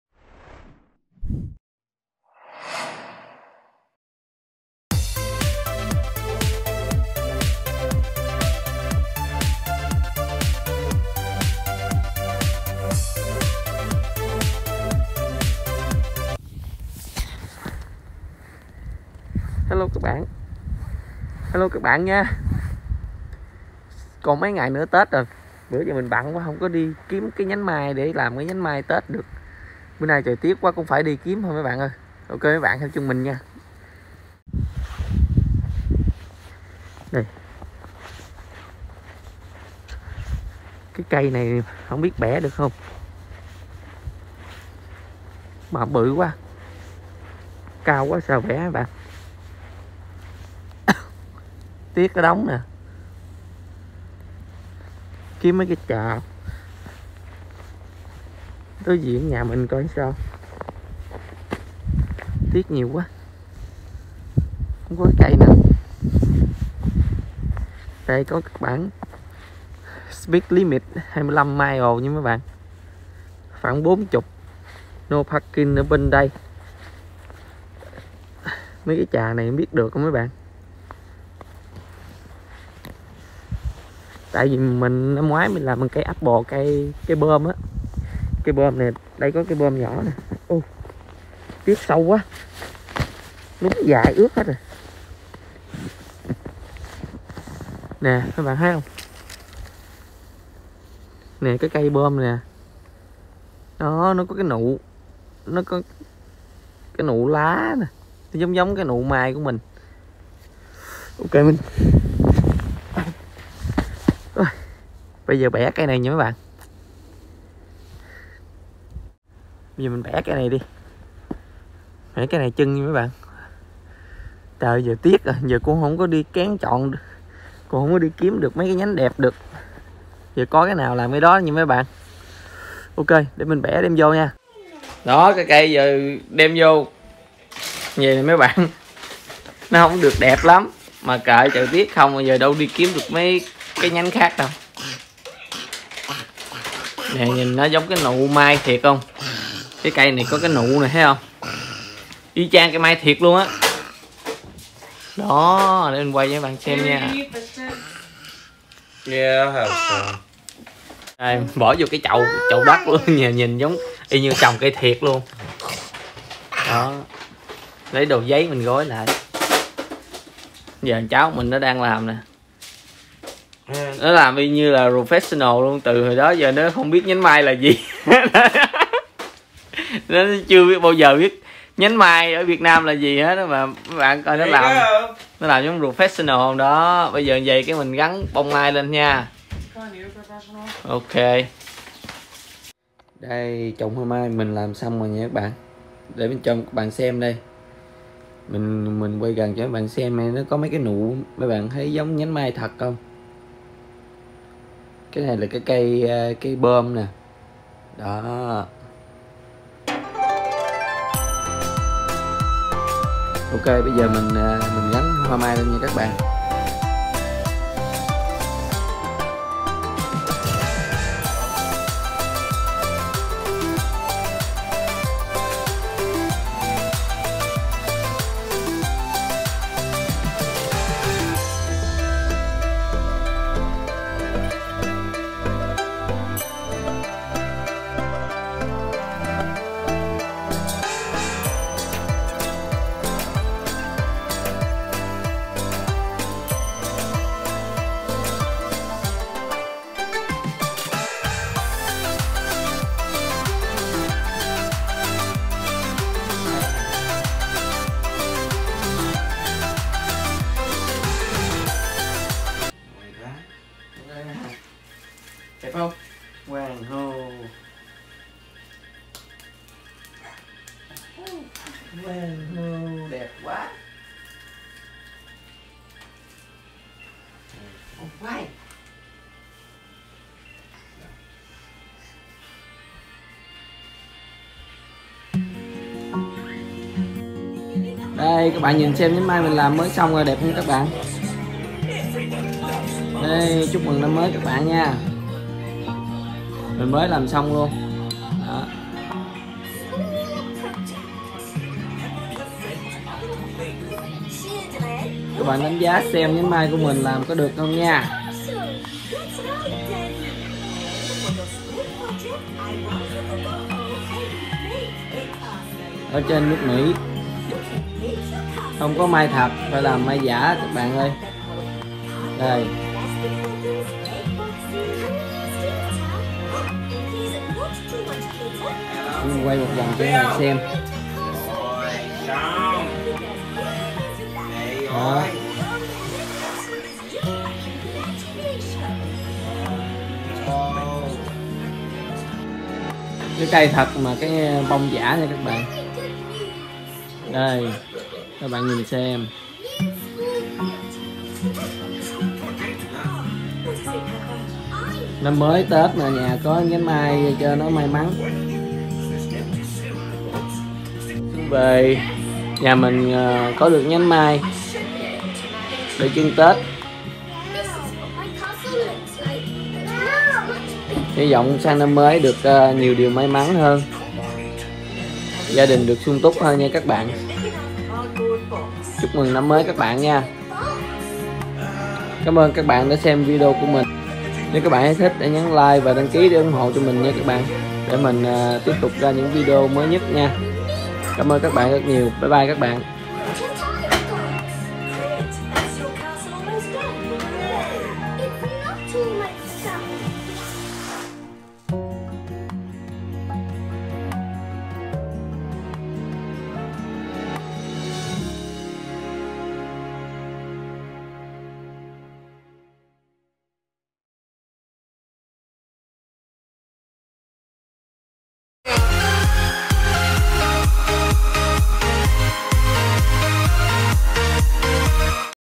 ANH DA ĐEN Lần đầu tiên làm nhánh mai chưng TẾT || USA TODAY
Rio Vlog USA
Chưng hoa mai ngày tết mang lại sự may mắn niềm vui tươi cho năm mới tràn đầy hạnh phúc!